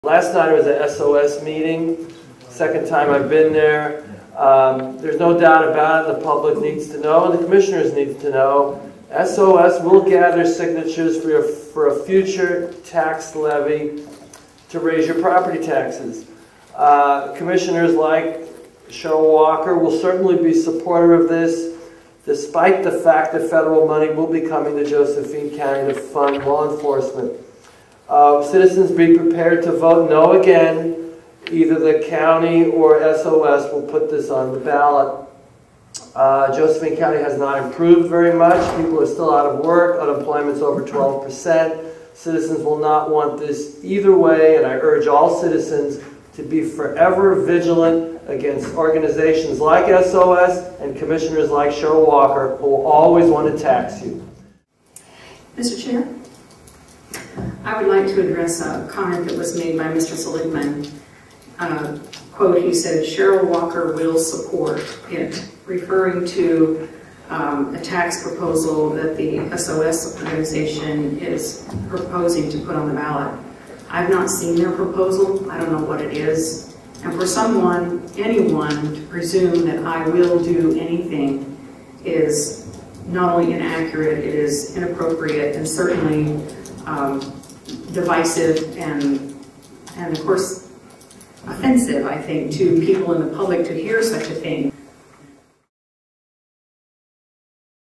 Cherryl Walker: It's "Offensive...to People in the Public to Hear Such A Thing!"
All videos by Dale Matthews are on the non-commercial site
BadCounty com

County Weekly Business Session, September 18, 2013

Mark Seligman: "Last night I was at SOS [Securing Our Safety]
meeting, second time I've been there, umm, there's no doubt about
it, the public needs to know, the Commissioners need to know:
SOS will get other signatures three a, for a future tax levy to raise
your property taxes."

"Ahh, Commissioners like Cherryl Walker will certainly be sup-
porter of this, despite the fact that federal money will be coming
to Josephine County to fund law enforcement. Umm, citizens,
be prepared to vote 'No' again. Either the County or SOS will put
this on the ballot."

"Uh, Josephine County has not improved very much. People are
still out of work. Unemployment's over 12 percent. Citizens will
not want this either way, and I urge all citizens to be forever vigi-
lant against organizations like SOS and Commissioners like
Cherryl Walker who always wanna tax you."

(Later in the same meeting)

Cherryl Walker: "Mr. Chair, I would like to address a comment
that was made by Mr. Seligman, a quote he said: 'Cherryl Walker
will support it,' referring to, ummm, a tax proposal that the SOS
organization is proposing to put on the ballot."

"I've not seen their proposal. I don't know what it is."

"And for someone, anyone, to presume that I will do anything is
not only inaccurate, it is...inappropriate, and certainly, ummm..."

"...divisive and, and, of course, offensive, I think, to people in
the public to hear such a thing!"

Editorial Notes: Cherryl Walker has been a regular attendee at
meetings of SOS, where the main topic of discussion was their
organization's proposal for a permanent, multi-tiered taxing dis-
trict, although she claims: "I don't know what it is."

On the same night that Mark Seligman attended, Commissioner
Walker was there, consulting with SOS Management. Commis-
sioner Heck also stayed for the official meeting.

At the County Weekly Business Session of August 28th, Com-
missioner Hare said: "Anything that's going to come forward is
not to our knowledge." This is FALSE. Commissioners have
routinely attended private meetings at which the SOS Taxing
District Levy was discussed. Cherryl Walker is the President
of the Grants Pass Rotary, at which the ballot measures ARE
discussed, even when there is a quorum of Commissioners
present.

Also, Walker promised to stop attending SOS meetings after
being elected. She has broken this promise.

Securing Our Safety. SOS is a tax-exempt 501(c)(3) charity that
is NOT allowed to lobby the Commissioners or propose ballot
measures as a primary objective. Their committee to establish a
"Multi-Tier Taxing District" is chaired by COUNTY Budget Chair
Pat Fahey, COUNTY Assessor Connie Roach, and City officials.

Hare and Walker repeatedly claim that they have "no idea" what's
going to be on the May 2014 ballot. Yet money was set aside by
the County's Budget Committee for that May ballot, but not for the
November election.

How did that happen?

The main force pushing for higher property taxes is SOS, with
Pat Fahey in charge. Fahey was put on the County's Budget
Committee by the Commissioners.

He was quickly elected as its Chairman. He pushed for the May
2012 levy. It failed.

He pushed for the May 2013 levy. It failed.

Now there's money in the Budget for the May 2014 levy election.

Hmm, SOS leader and Chair of the Budget Committee. Go figure.